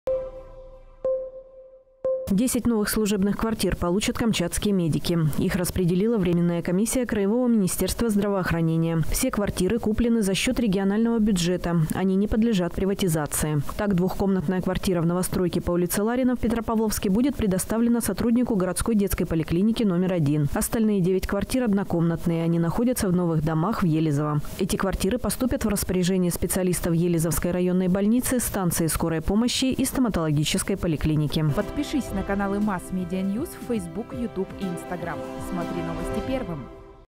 Новые служебные квартиры для медиков
Десять новых служебных квартир получит камчатские медики. Их  распределила временная комиссия краевого министерства здравоохранения. Все квартиры куплены за счет регионального бюджета, они не подлежат приватизации. Так, двухкомнатная квартира в новостройке по ул. Ларина в Петропавловске будет предоставлена сотруднику городской детской поликлиники №1. Остальные девять квартир – однокомнатные. Они находятся в новых домах на ул. Гришечко в Елизове. Эти квартиры поступят в распоряжение специалистов елизовских районной больницы, станции скорой помощи и стоматологической поликлиники.